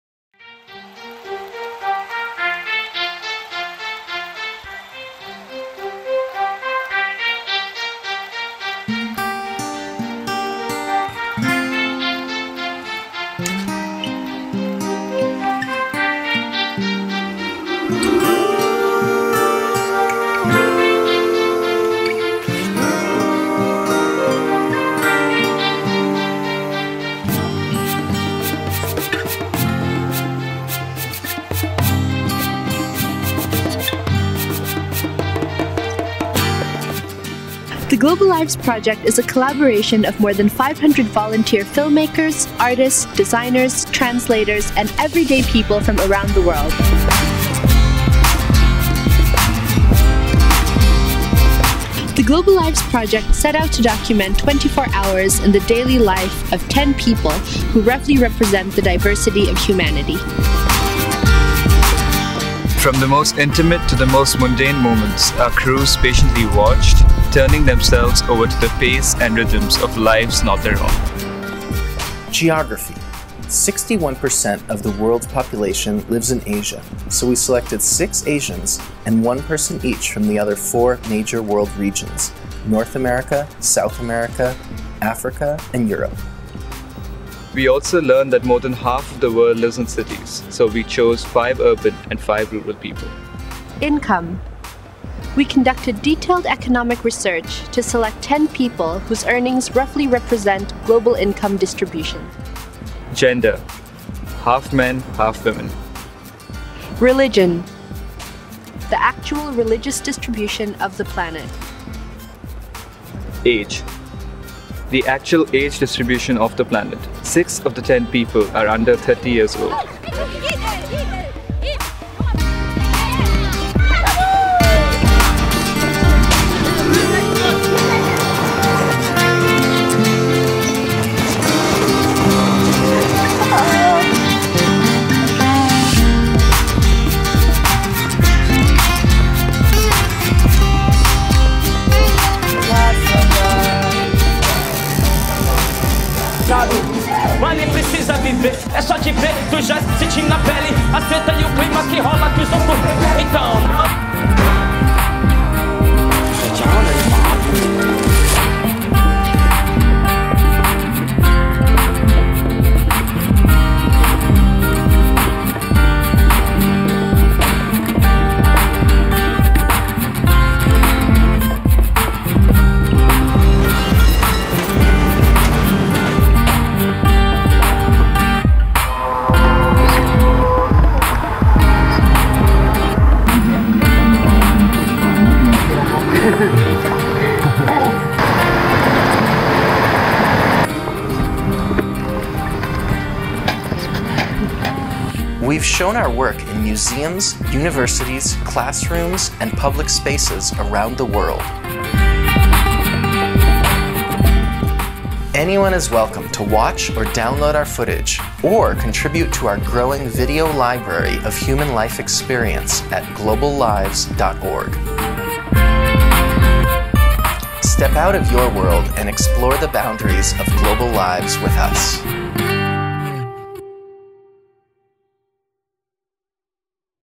Global Lives Project
The Global Lives Project is a collaboration of filmmakers, photographers, artists and everyday people working together to create a video library of human life experience. The show features 240 hours of video of Brazil, Malawi, Japan, China, Indonesia, India, Serbia, Lebanon, Kazakhstan and the US.